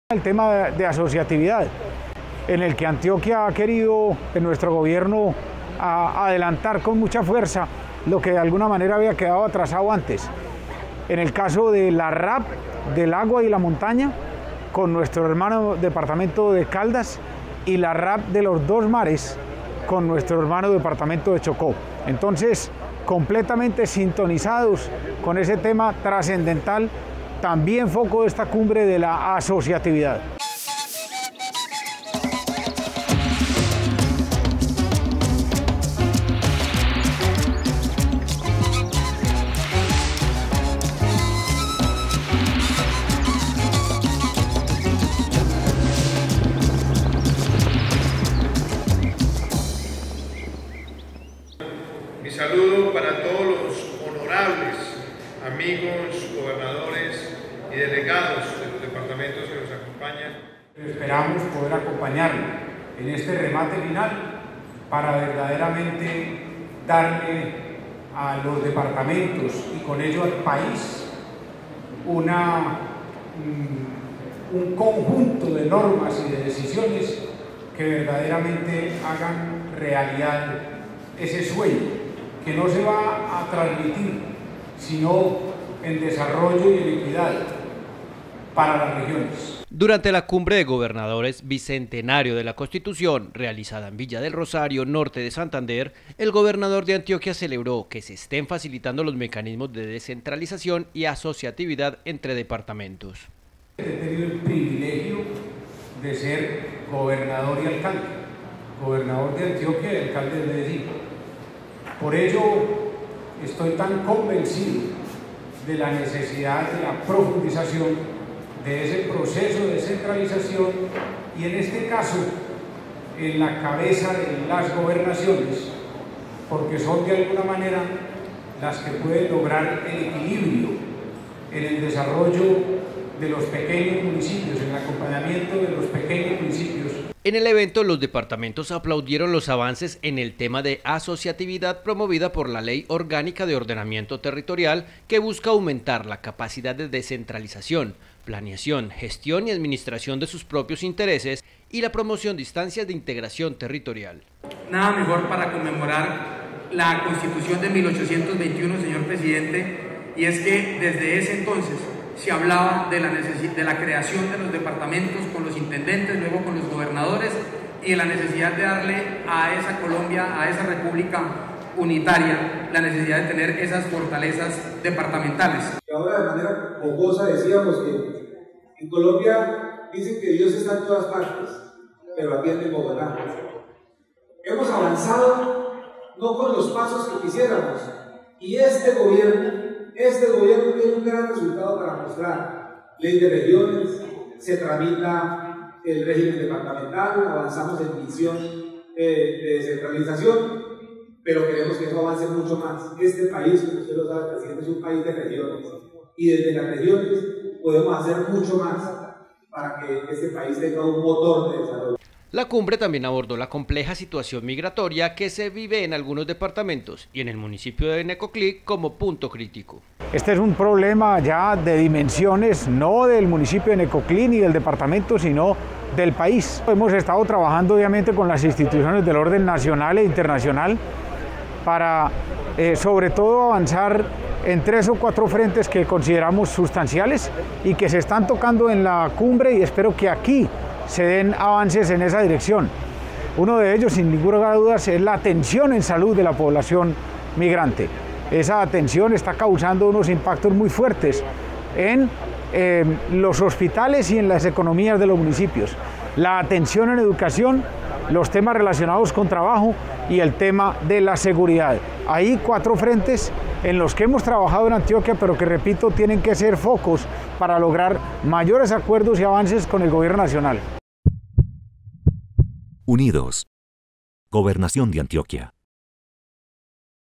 UNIDOSConElGobernador Cumbre de gobernadores
UNIDOS, Gobierno Nacional y departamentos, tenemos el compromiso de equilibrar el desarrollo en todos los territorios. En la CumbreBicentenario dialogamos sobre migración, seguridad y asociatividad.